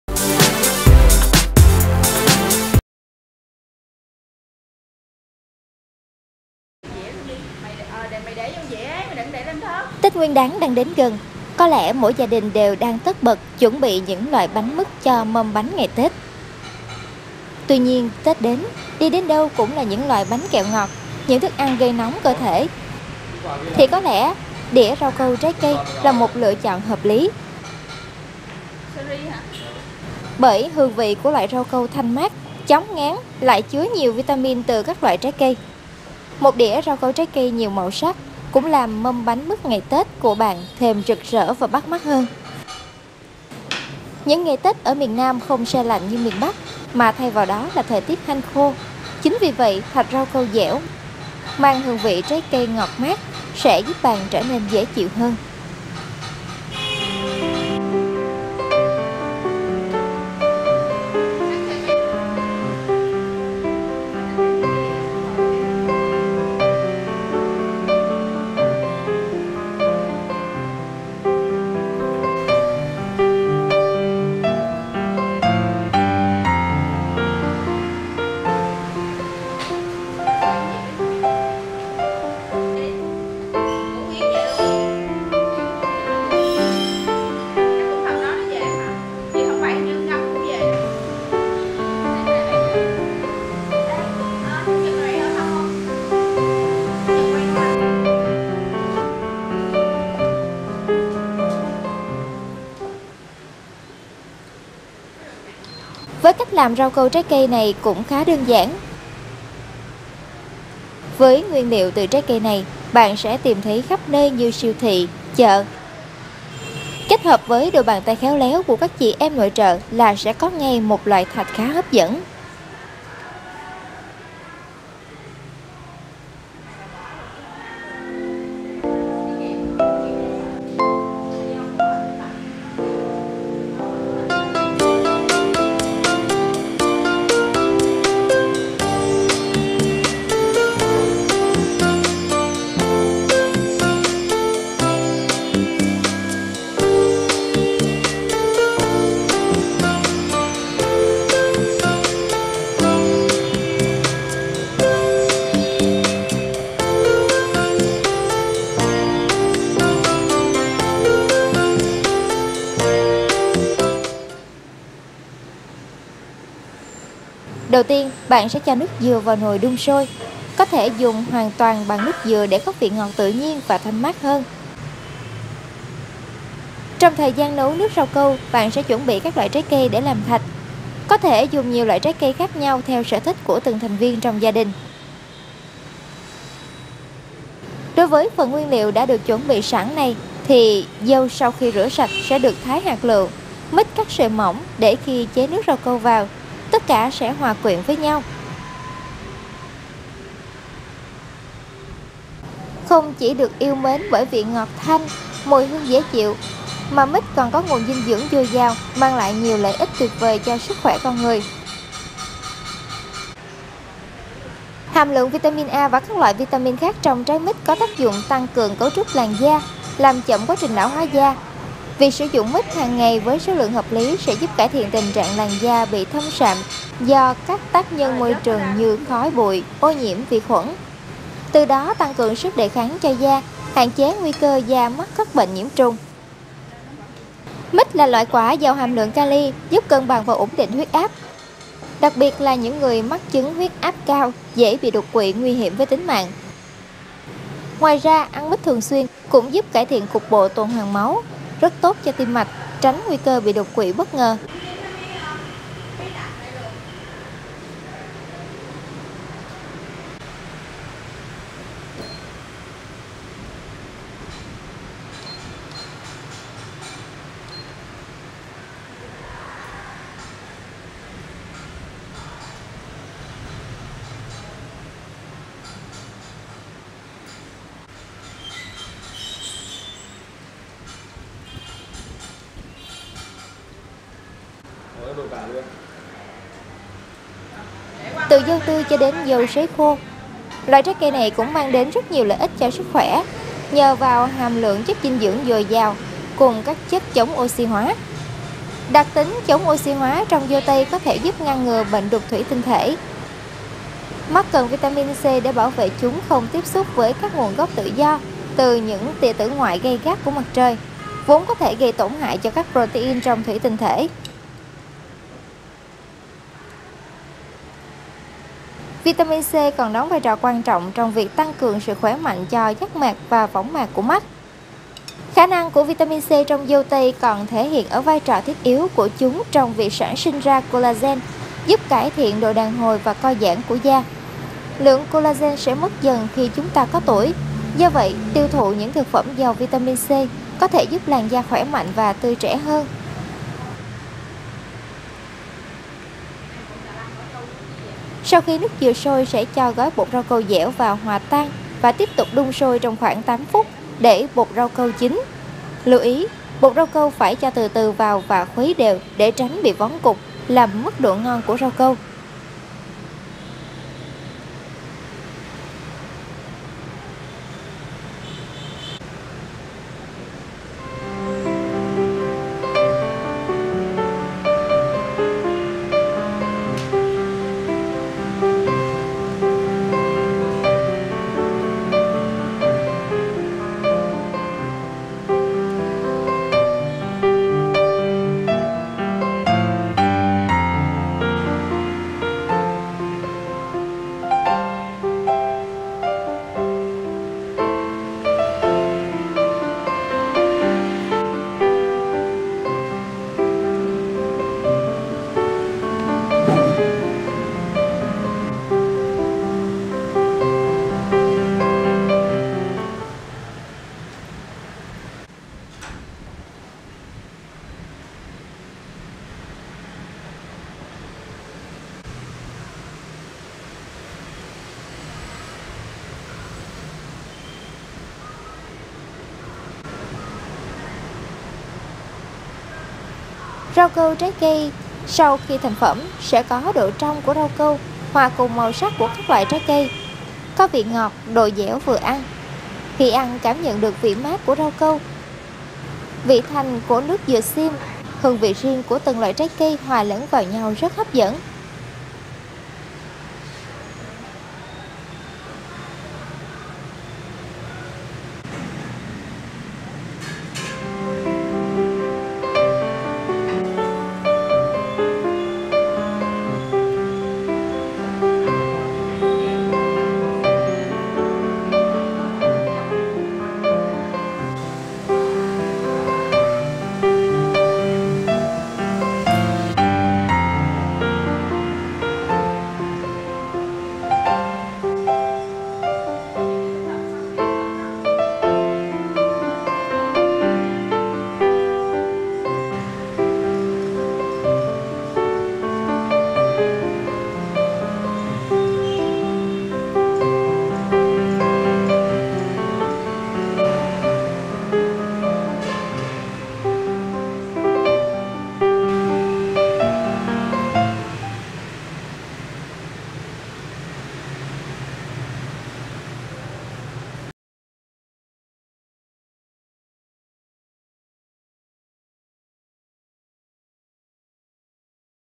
Ngày tết thanh mát với cách làm thạch rau câu trái cây
Tết Nguyên đán đến gần, có lẽ mỗi gia đình đều đang tất bật chuẩn bị những loại

bánh mứt cho mâm bánh ngày Tết. Tuy nhiên Tết đến, đi đến đâu cũng là những loại

bánh kẹo ngọt, những thức ăn gây nóng cơ thể thì có lẽ đĩa rau câu trái cây là một

lựa chọn hợp lý. Bởi hương vị của loại rau câu thanh mát, chóng ngán lại chưá nhiều

vitamin từ các loại trái cây.

Một đĩa rau câu trái cây nhiều màu sắc cũng làm mâm bánh mứt ngày tết của bạn

thêm rực rỡ và bắt mắt hơn.

Những ngày Tết ở miền Nam không se lạnh như miền Bắc, mà thay vào đó là thời tiết

hanh khô, chính vì vậy thạch rau câu dẻo mang hương vị trái cây ngọt mát sẽ giúp

bạn trở nên dễ chịu hơn.Với cách làm rau câu trái cây này cũng khá đơn giản. Với nguyên liệu từ trái cây này

bạn sẽ tìm thấy khắp nơi như siêu thị, chợ,…kết hợp với đôi bàn tay khéo léo của các

chị em nội trợ là sẽ có ngay một loại thạch khá hấp dẫn.Đầu tiên bạn sẽ cho nước dừa vào nồi đun sôi. Có thể dùng hoàn toàn bằng nước dừa

để có vị ngọt tự nhiên và thanh mát hơn. Sau khi nước dừa sôi sẽ cho gói bột rau câu

dẻo vào hòa tan và tiếp tục đun sôi trong khoảng 8 phút để bột rau câu chín. Lưu ý,

bột rau câu phải cho từ từ vào và khuấy đều để tránh bị vón cục, làm mất độ gnon

của rau câu.Trong thời gian nấu nước rau câu bạn sẽ chuẩn bị các loại trái cây để làm thạch. Có

thể dùng nhiều loại trái cây khác nhau theo sở thích của từng thành viên trong gia

đình.

Đối với phần nguyên liệu đã được chuẩn bị sẵn này thì dâu sau khi rửa sạch sẽ được

thái hạt lựu, mít cắt sợi mỏng để khi chế nước rau câu vào tất cả sẽ hòa quyện với

nhau.

Điểm cộng của loại rau câu trái cây là các nguyên liệu hoàn toàn tự nhiên, trái cây

không qua sơ chế tránh làm hao hụt các vitamin.Dù được sử dụng dưới bất kỳ hình thức nào, từ dâu tươi cho đến dâu sấy khô, loại

trái cây này cũng mang đến rất nhiều lợi ích cho sức khỏe nhờ vào hàm lượng chất

dinh dưỡng dồi dào cùng các chất chống oxy hóa. Đặc tính chống oxy hóa trong dâu

tây có thể giúp ngăn ngừa bệnh đục thủy tinh thể. Mắt cần vitamin C để bảo vệ chúng

không tiếp xúc với các gốc tự do từ những tia tử ngoại gay gắt của mặt trời, vốn có

thể gây tổn hại cho các protein trong tủy tinh thể. Vitamin C còn đóng vai trò quan

trọng trong việc tăng cường sự khỏe mạnh cho giác mạc và võng mạc của mắt.

Khả năng của vitamin C trong dâu tây còn thể hiện ở vai trò thiết yếu của chúng

trong việc sản sinh ra collagen, giúp cải thiện độ đàn hồi và co giãn của da. Lượng

collagen sẽ mất dần đi khi chúng ta có tuổi, do vậy, tiêu thụ những thực phẩm giàu

vitamin C có thể giúp làn da khỏe mạnh và tươi trẻ hơn.

Không chỉ được yêu mến với vị ngọt thanh, mùi hương dễ chịu mà mít còn có nguồn

dinh dưỡng dồi dào, mang lại nhiều lợi ích tuyệt vời cho sức khỏe con người.

Hàm lượng vitam A và các loại vitamin khác trong trái mít có tác dụng tăng cường

cấu trúc làn da, làm chậm quá trình lão hóa da. Việc sử dụng mít hàng ngày với số

lượng hợp lý sẽ giúp cải thiện tình trạng làn da bị thâm sạm do các tác nhân môi

trường như khói bụi, ô nhiễm, vi khuẩn…Từ đó tăng cường sức đề kháng cho da, hạn

chế nguy cơ da mắc các bệnh nhiễm trùng.Mít là loại quả giàu hàm lượng kali, giúp cân bằng và ổn định huyết áp, đặc biệt là

những người mắc chứng huyết áp cao dễ bị đột quỵ nguy hiểm tới tính mạng.

Ngoài ra, ăn mít thường xuyên cũng giúp cải thiện cục bộ tuần hoàn máu, rất tốt cho

tim mạch, tránh nguy bị cơ đột quỵ bất ngờ.Rau câu sau khi thành phẩm sẽ có độ trong của rau câu hòa cùng màu sắc của các

loại trái cây. Có vị ngọt, độ dẻo vừa ăn. Khi ăn cảm nhận được vị mát của rau câu, vị

thanh của nước dừa xiêm, hương vị riêng của từng loại trái cây hòa lẫn vào nhau rất

hấp dẫn.